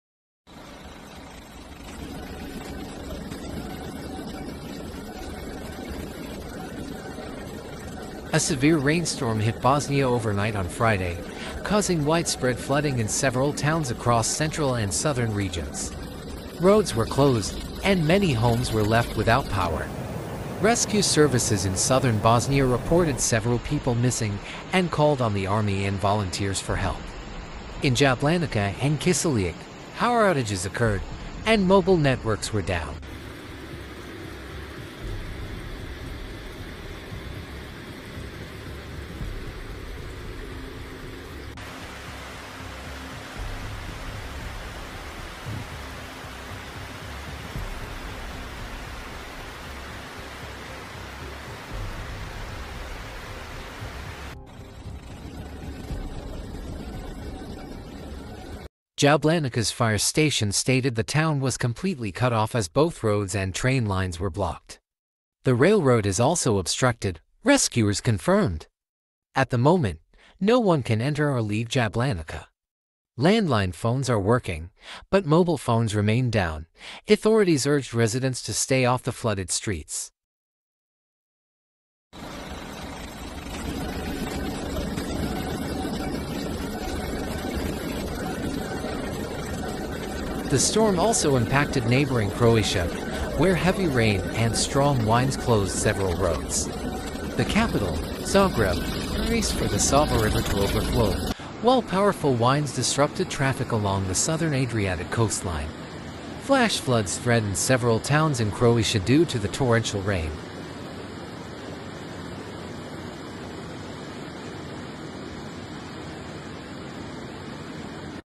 Bosnia Underwater: Severe Flooding Shuts Down Towns and Railways
A severe rainstorm hit Bosnia overnight on Friday, causing widespread flooding in several towns across central and southern regions. Roads were closed, and many homes were left without power. Rescue services in southern Bosnia reported several people missing and called on the army and volunteers for help. In Jablanica and Kiseljak, power outages occurred, and mobile networks were down.

Jablanica’s Fire Station stated the town was completely cut off as both roads and train lines were blocked. "The railroad is also obstructed," rescuers confirmed. "At the moment, no one can enter or leave Jablanica. Landline phones are working, but mobile phones remain down." Authorities urged residents to stay off the flooded streets.

The storm also impacted neighboring Croatia, where heavy rain and strong winds closed several roads. The capital, Zagreb, braced for the Sava River to overflow, while powerful winds disrupted traffic along the southern Adriatic coastline. Flash floods threatened several towns in Croatia due to the torrential rain.

weather wev